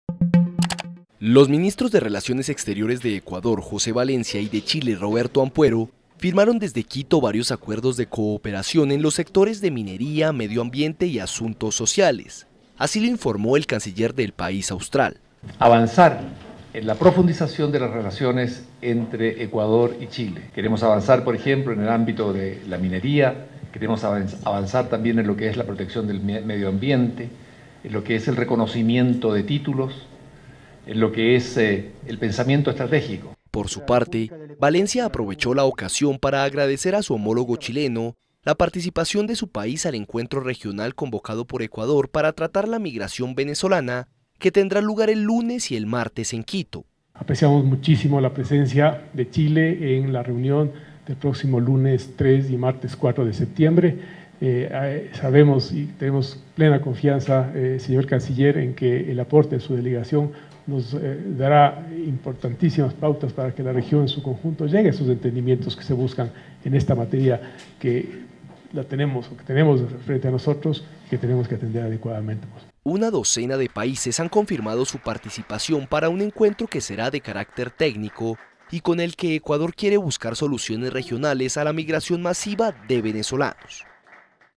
Ecuador y Chile rubrican acuerdos bilaterales en temas ambientales, mineros y sociales
Quito, 31 ago (EFE),(Imágenes: Juan Francisco Chávez).- El ministro de Relaciones Exteriores de Ecuador, José Valencia, y su homólogo chileno, Roberto Ampuero, firmaron hoy en Quito varios acuerdos de cooperación en los sectores de minería, medioambiente y asuntos sociales.